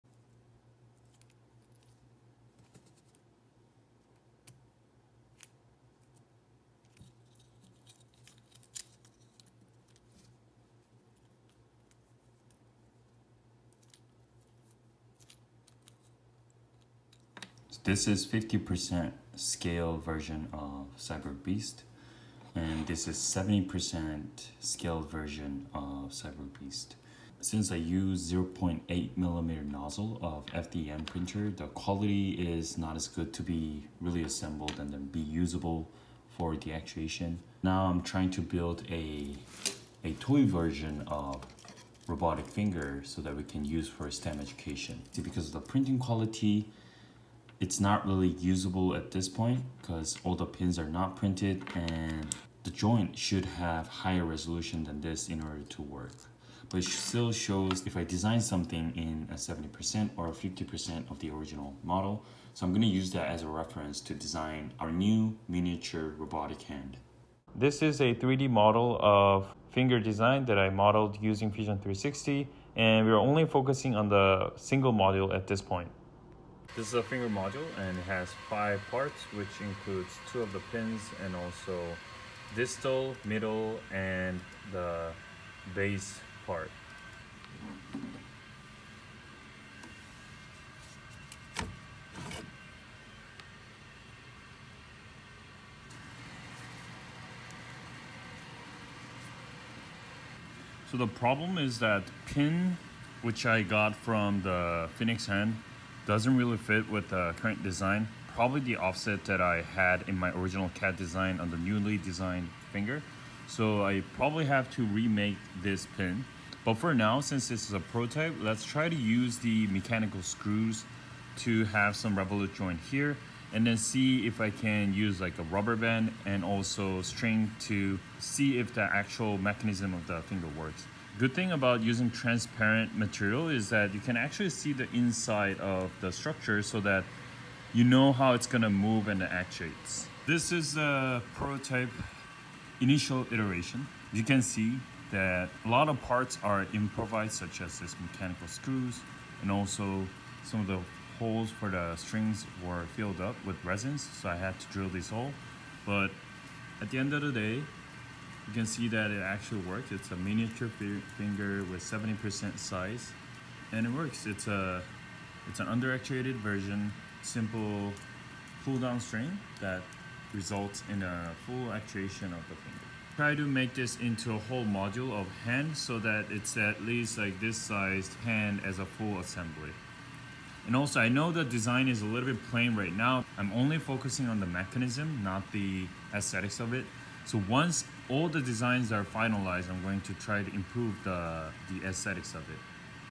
Building a Mini Robotic Hand from Scratch with 3D Printing
New project alert! Mini robotic hand kit for STEM education.